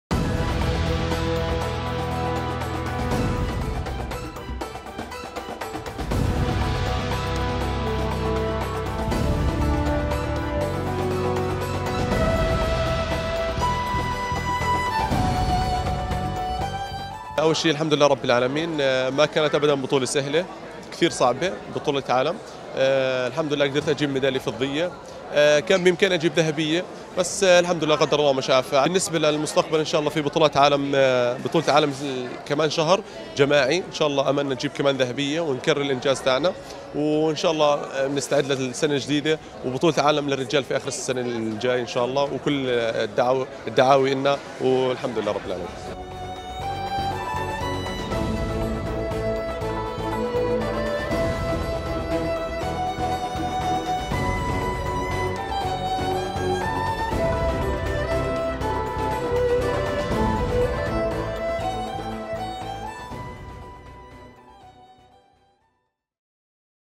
"الجعفري" يُحقق الفضية في ختام بطولة العالم للكراتيه
محمد الجعفري يظفر بالميدالية الفضية ومنتخبنا الوطني يُنهي مشاركته بـ ٣ ميداليات في بطولة العالم للكراتيه للناشئين والشباب وتحت ٢١ عاماً 🥇🥈🥈